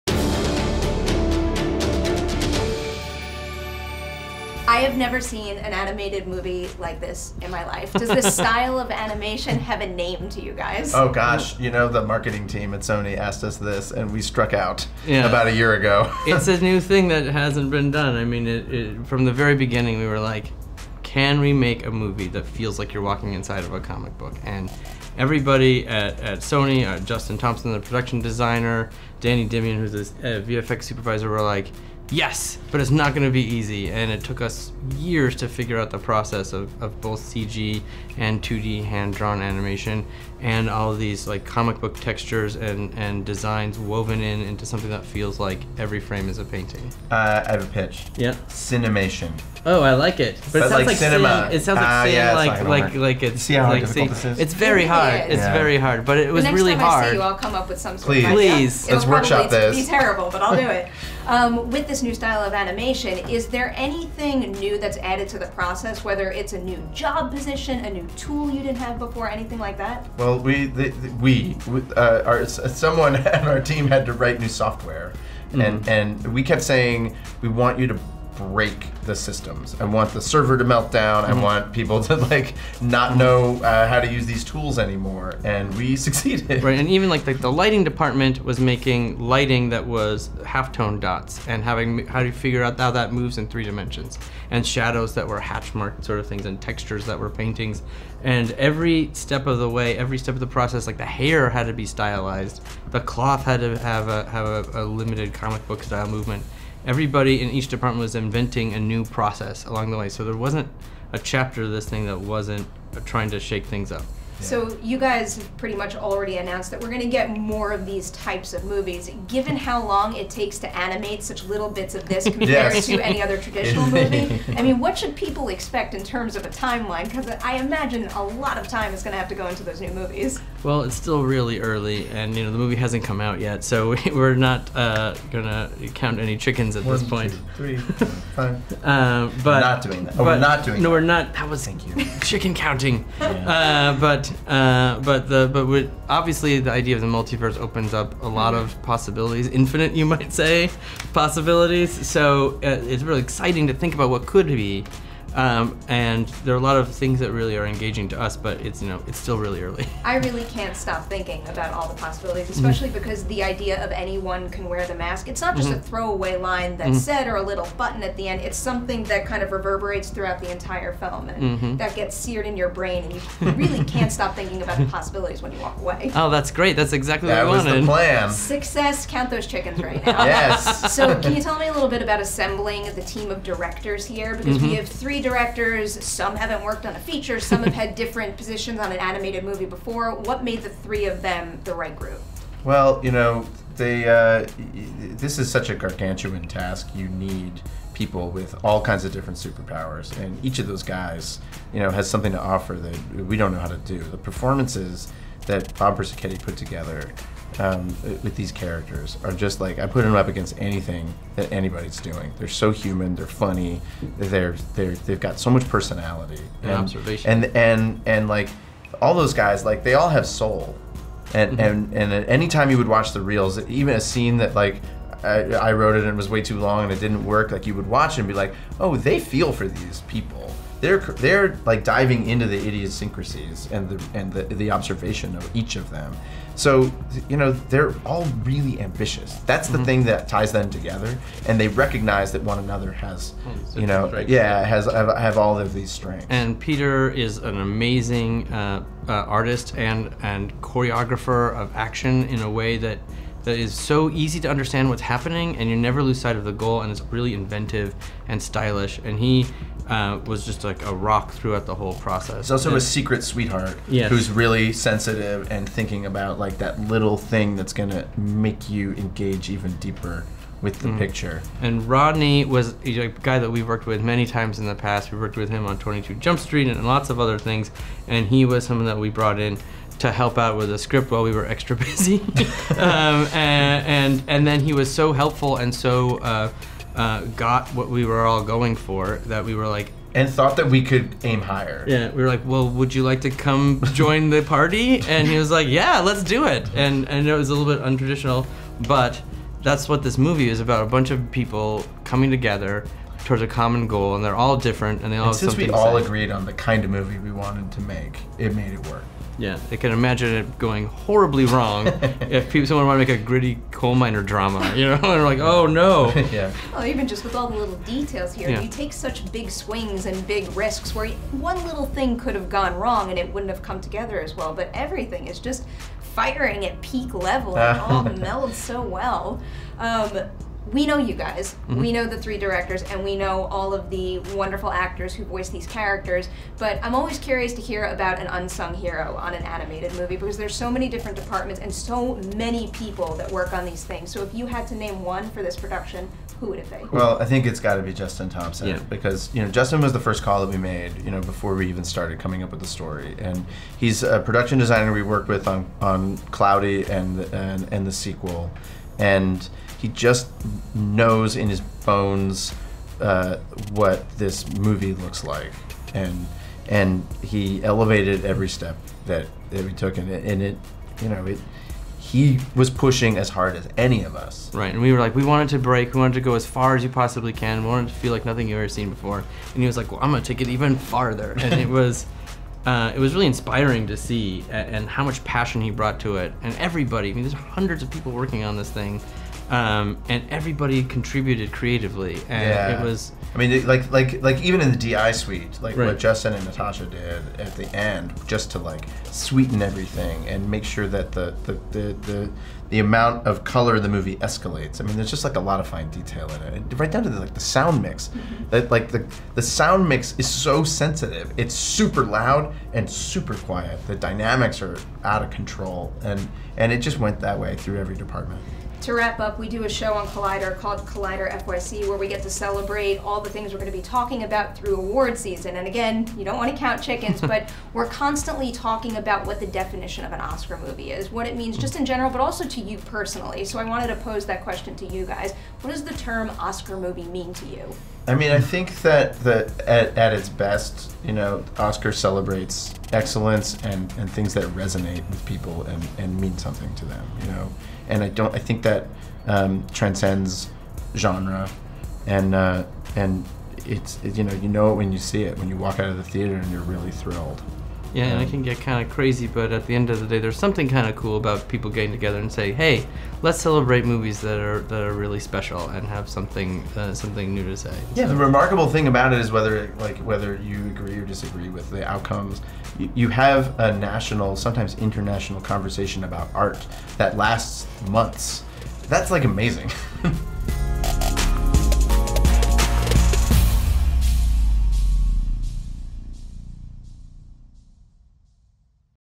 Spider-Man: Into the Spider-Verse: Phil Lord and Chris Miller Interview
Get ready for Spider-Man: Into the Spider-Verse. Not only is the film a fantastic celebration of Spider-Man and what it means to wear the mask, but it also offers up a hilarious and heartfelt story of a teenager trying to find his place and embrace what makes him special. And then, on top of all that, the film also marks a major breakthrough for animation. You know all that talk about the movie feeling like a comic book? It's true! In fact, the team behind the film takes that technique above and beyond, with every single frame of the movie beautifully designed with unprecedented style and purpose.

In case you couldn't tell, I love the movie, so getting the opportunity to sit down with producer Chris Miller and write-producer Phil Lord was a real treat. I suspect no amount of time would be good enough to cover the entirety of this animation process, but we did get the chance to talk about the new tools developed to bring Into the Spider-Verse to screen, assembling the trio of directors, and who Lord and Miller would name the "unsung heroes" of the production. You can hear about all of that in this video interview and if you're looking for even more on the making of the film, be sure to keep an eye out for cast interviews as well as the full Q&A from our recent screening at the ArcLight Cinemas Hollywood with Lord, Miller and directors Bob Persichetti, Peter Ramsey and Rodney Rothman.

Spider-Man: Into the Spider-Verse hits theaters nationwide on December 14th.